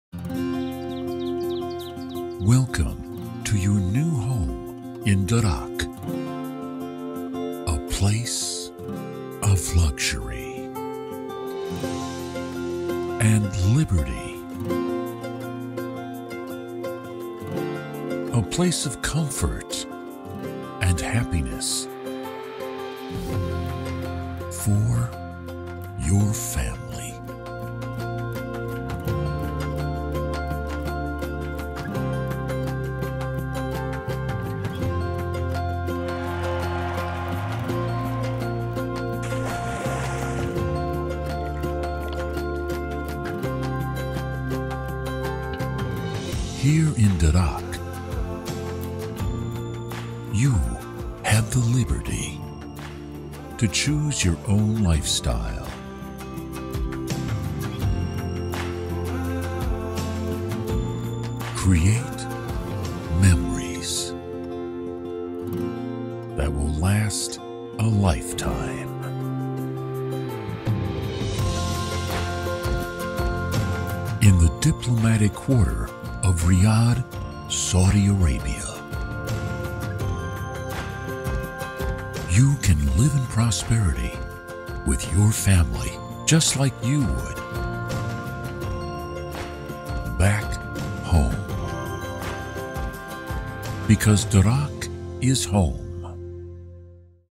Darraq is home
Delve into the world of luxury living and join Riyadh’s most vibrant  community, set against the prestigious backdrop of the Diplomatic Quarter.
Darraq’s creative blend of urban architecture, smart home technology and timeless beauty offer the epitome
of lavish and modern living.